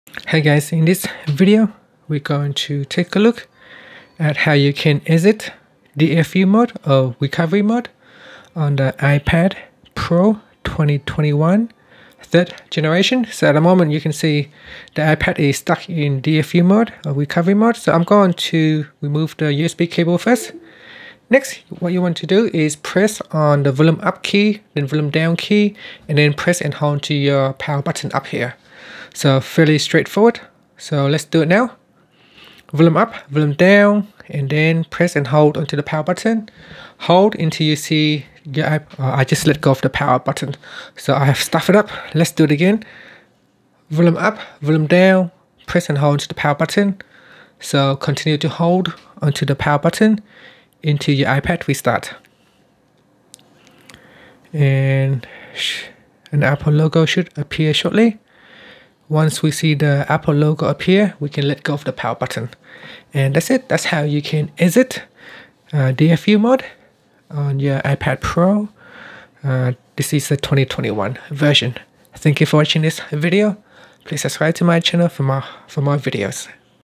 iPad Pro 2021: How to Exit DFU Mode (Device Firmware Update) / Recovery Mode
Learn how you can exit DFU mode(Device Firmware Update) / Recovery Mode on iPad Pro 2021.